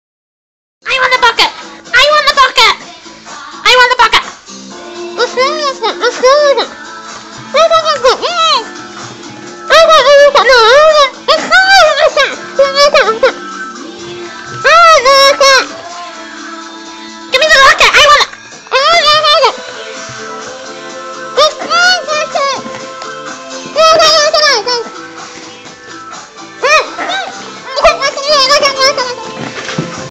◄▓▓ Listen to 4 Alaskan malamute puppies fighting over a bucket! LOL! ▓▓►►
Here are 4 of our Alaskan Malamute puppies playing and fighting over a bucket!  for all our kennel info :o)